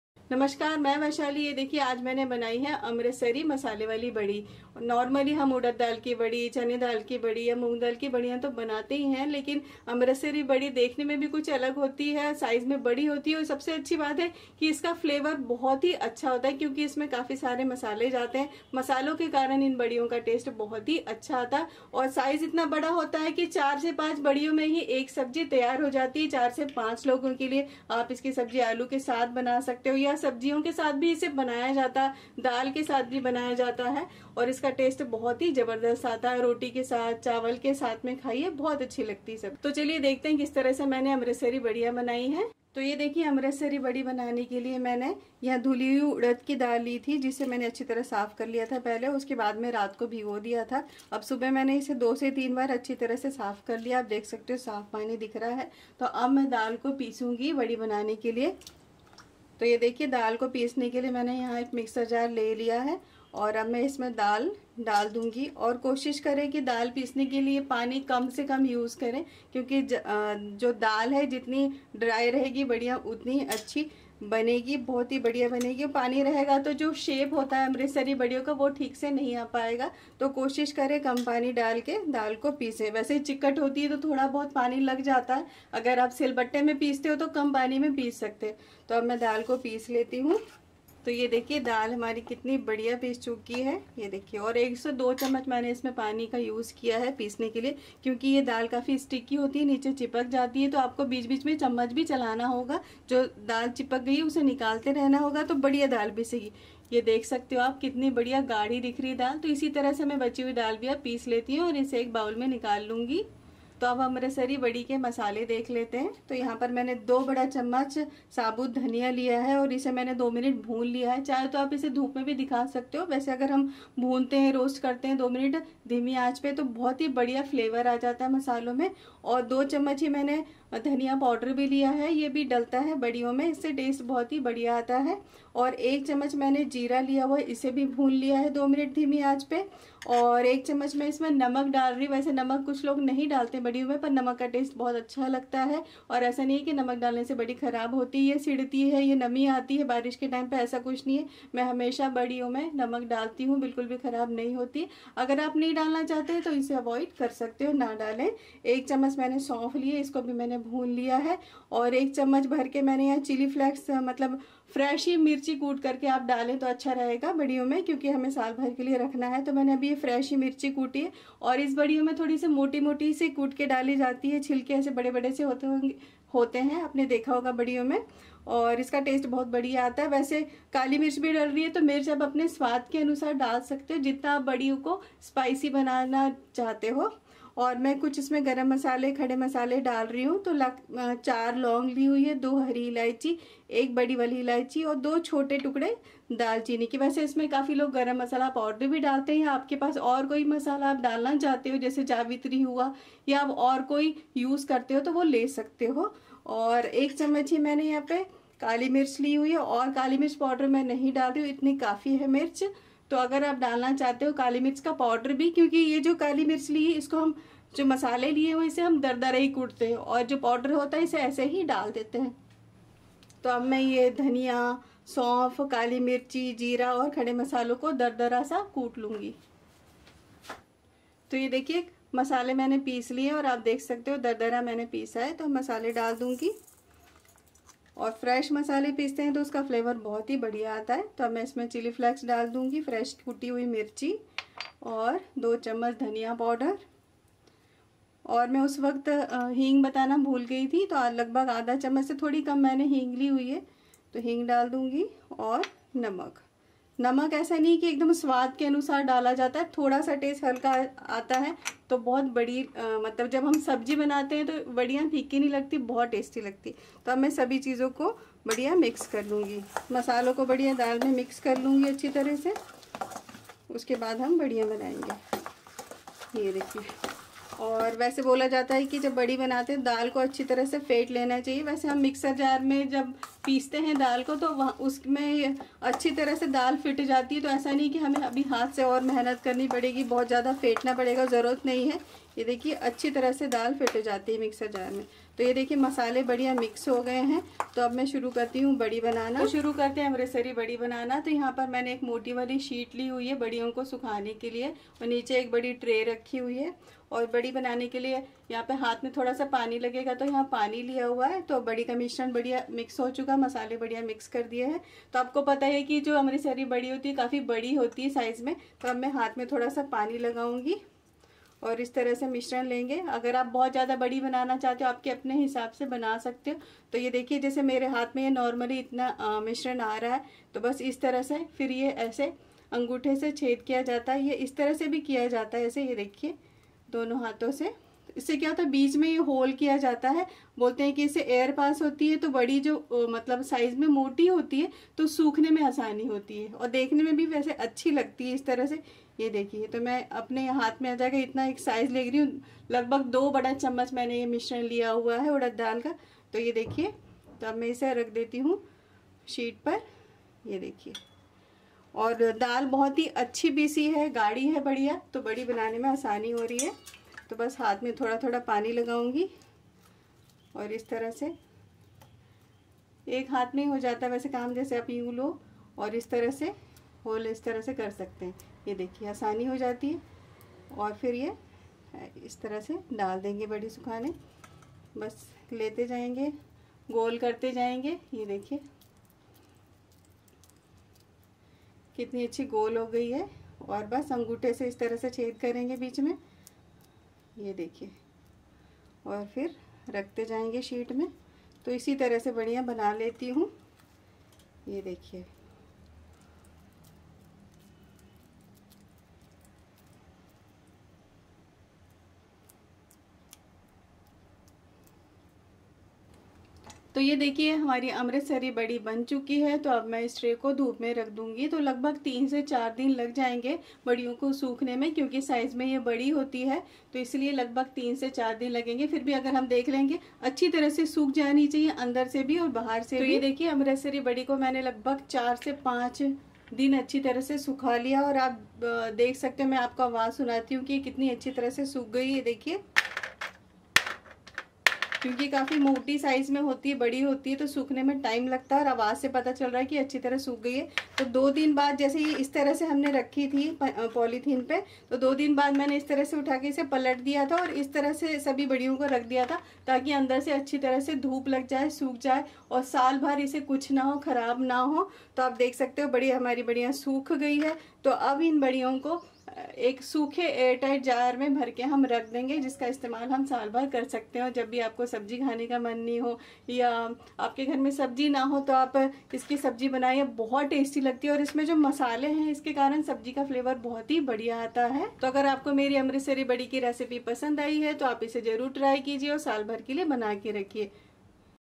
घर पर अमृतसरी वडियां बनाने का आसान तरीका - Amritsari Wadi Recipe - how to make Punjabi Wadiyan
Masala Urad Dal Vadi or Amritsari Wadiyan is an authentic Punjabi dish. Use this vadi with any curry recipe especially with Potato and enjoy!!!